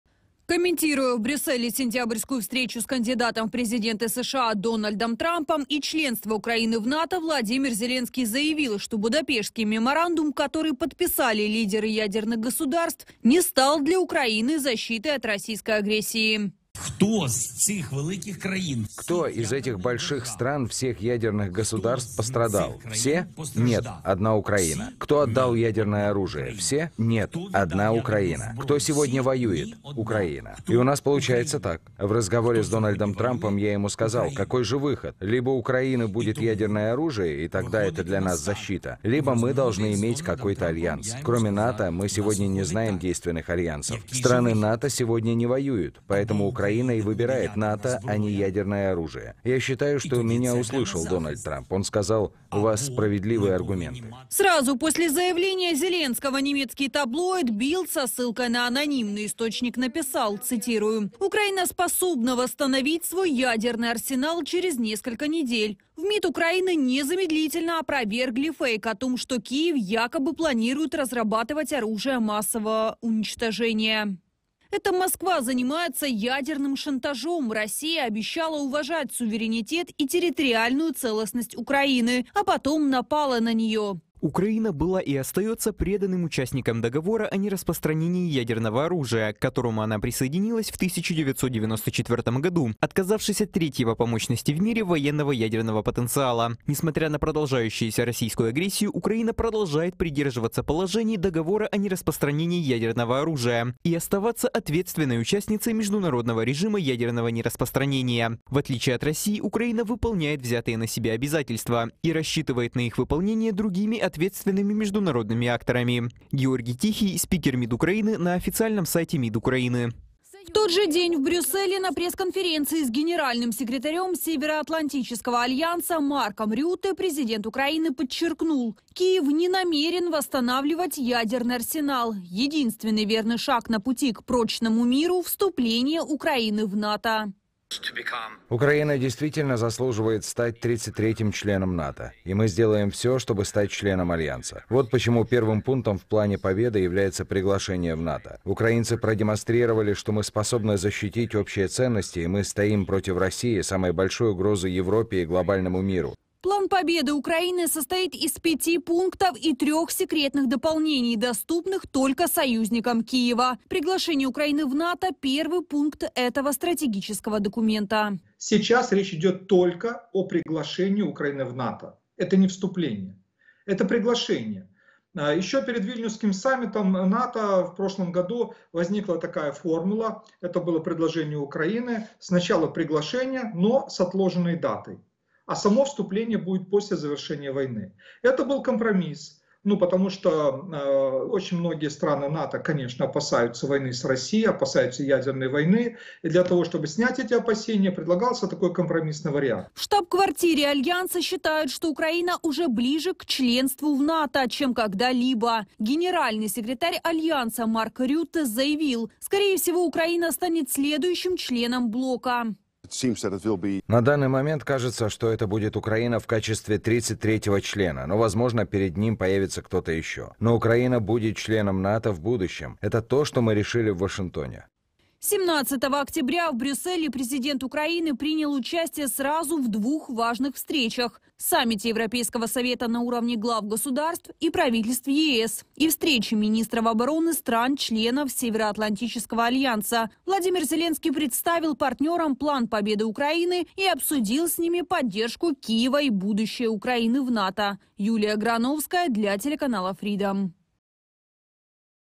ОГО! Зеленский ВЫСТАВИЛ ЗАПАДУ УЛЬТИМАТУМ! ЯДЕРКА или НАТО: что спасет Украину НА САМОМ ДЕЛЕ?
“Либо НАТО, либо ядерное оружие” – главный меседж Зеленского прозвучал накануне. Готовится ли Украина создавать ядерное оружие? Что еще заявил президент Украины? И что действительно поможет Украине? Все детали – в нашем видео.